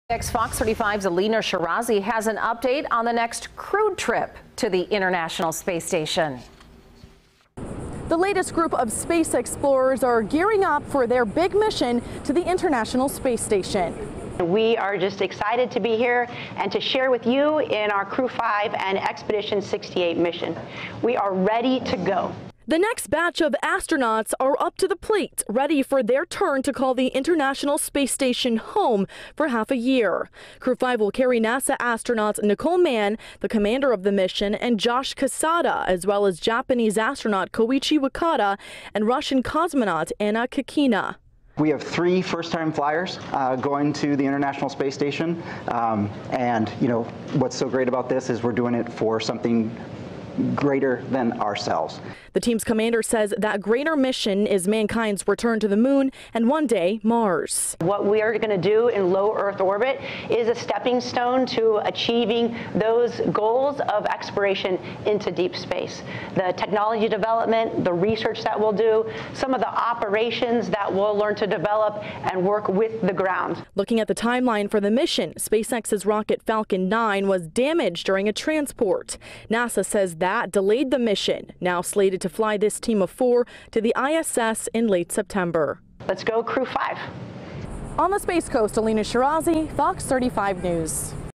SpaceX preparing for next crewed mission
Four astronauts from three countries are scheduled to launch on the SpaceX Crew-5 mission to the International Space Station this fall, and for the first time since NASA began launching from U.S. soil again, a Russian cosmonaut will be joining the crew.